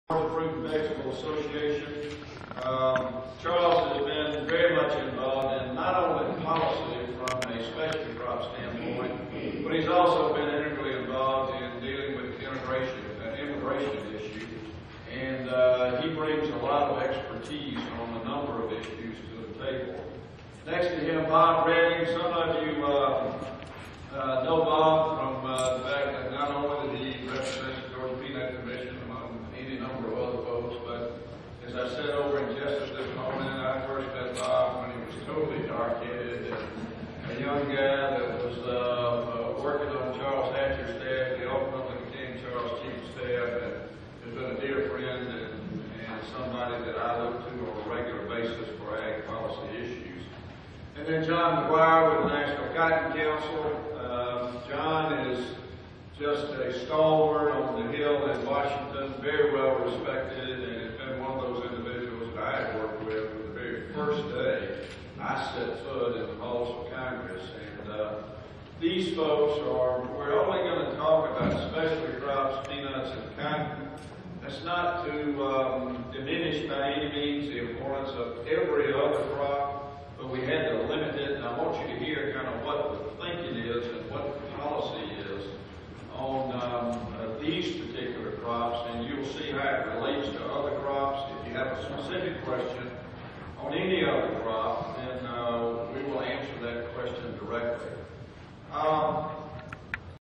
Introduction of panelists @ Farm Bill 2012-03-16
Farm Bill Forum, Senator Saxby Chambliss,
Gary Black, Georgia Commissioner of Agriculture,
Charles Hall, Georgia Fruit and Vegetable Growers Association,
Robert Redding, Southern Peanut Farmers Federation,
John Maguire, National Cotton Council,
UGA Tifton Campus Conference Center, Tifton, Tift County, Georgia, 16 March 2012.